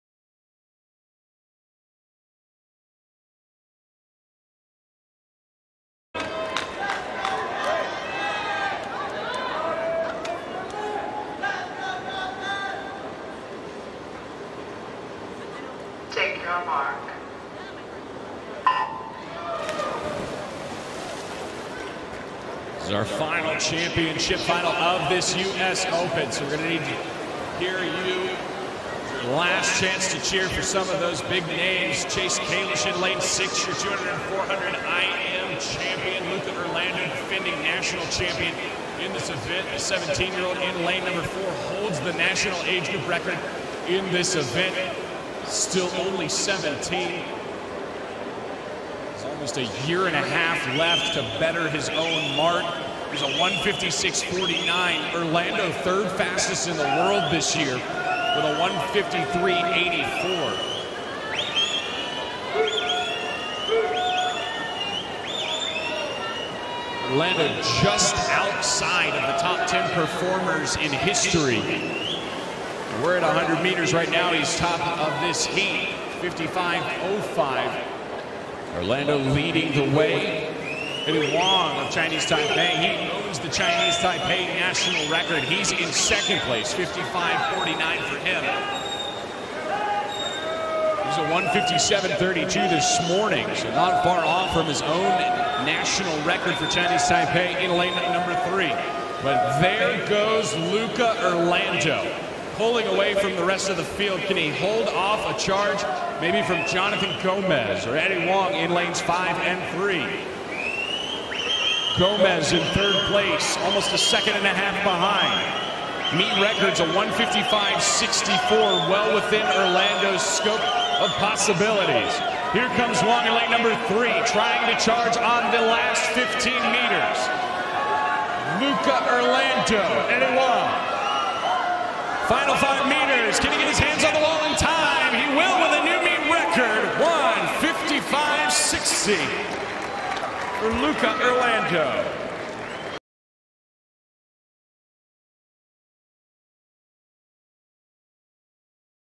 17 Year Old Luca Urlando vs Jonathan Gomez | Men’s 200m Fly A Final
At the Men’s 200m Fly A Final, the 2019 Toyota U.S. Open sees Luca Urlando compete against Eddie Wang and Jonathan Gomez.